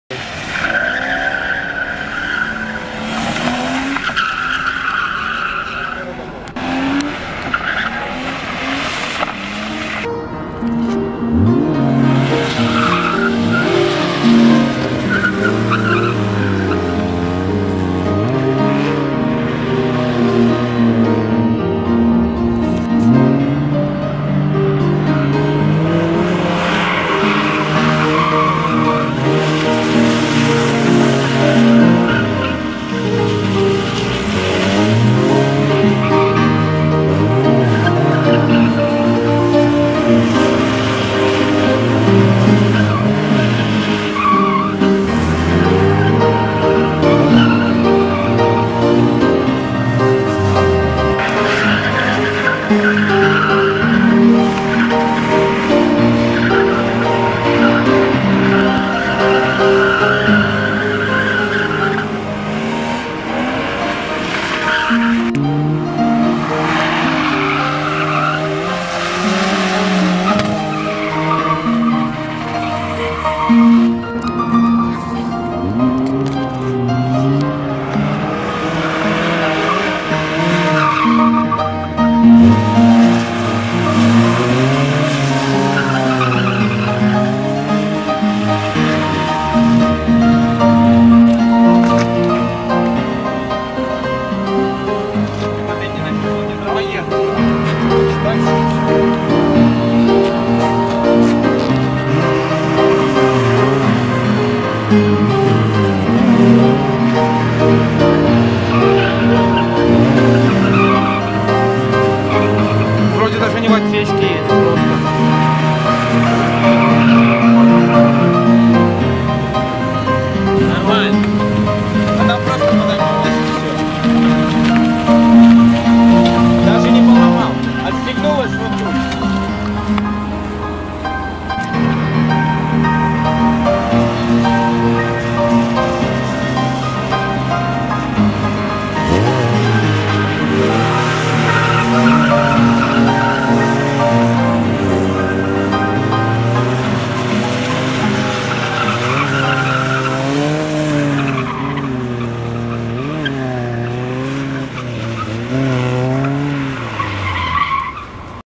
пескобаза Жигули юного Стилова и е36
Летняя архивная вылазка на пескобазу, проба первых доработок е36